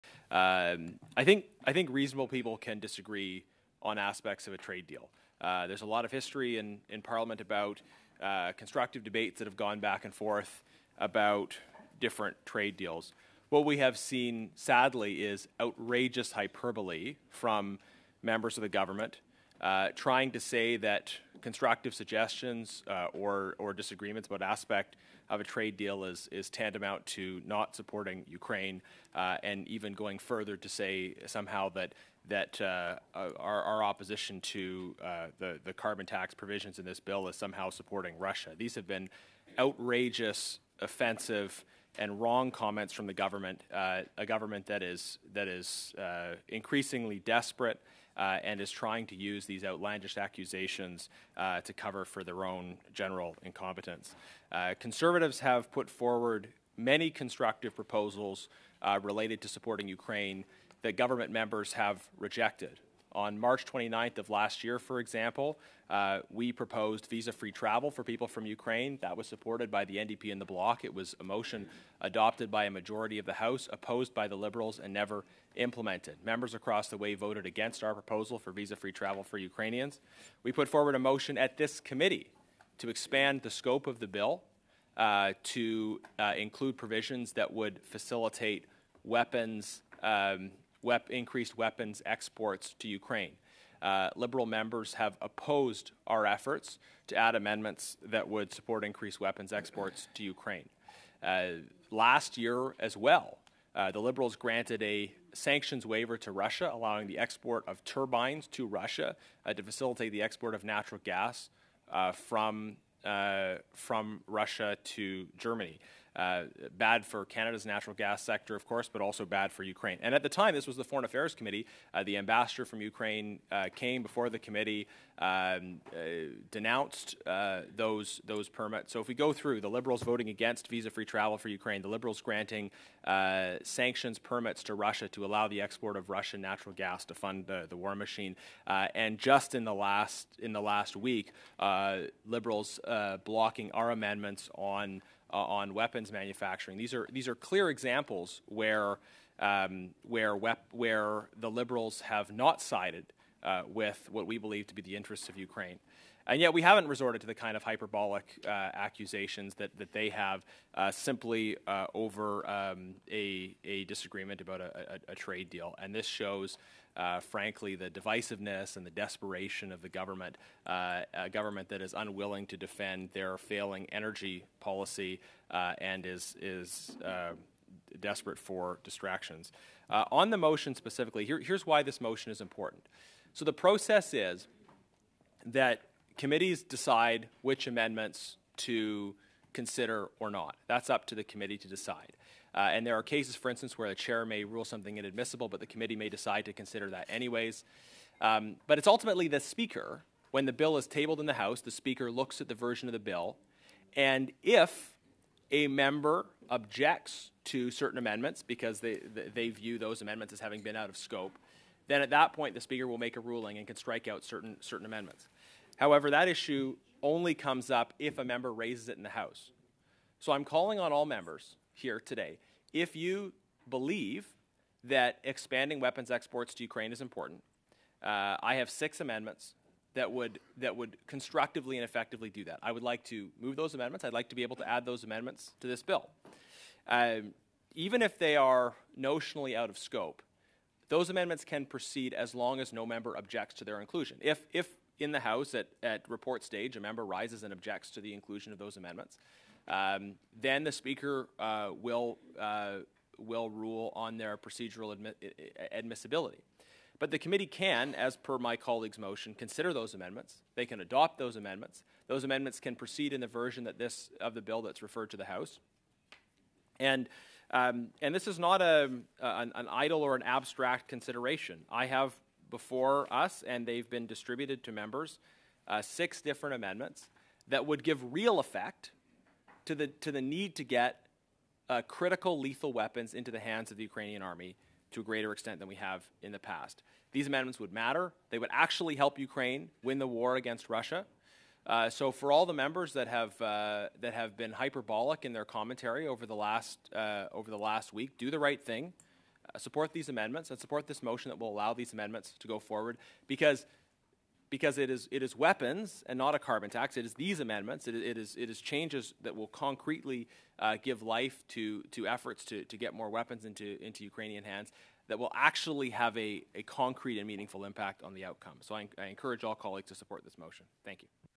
Increasingly DESPERATE Liberal government is trying to cover for its own INCOMPETENCE
"These are clear examples of how the Liberals have not sided with what we believe to be the interests of Ukraine, and yet we haven't resorted to the kinds of hyperbolic accusations they have simply over a disagreement about a trade deal.
This shows, frankly, the divisiveness and the desperation of the government, a government that is unwilling to defend its failing energy policy and that is desperate for distractions."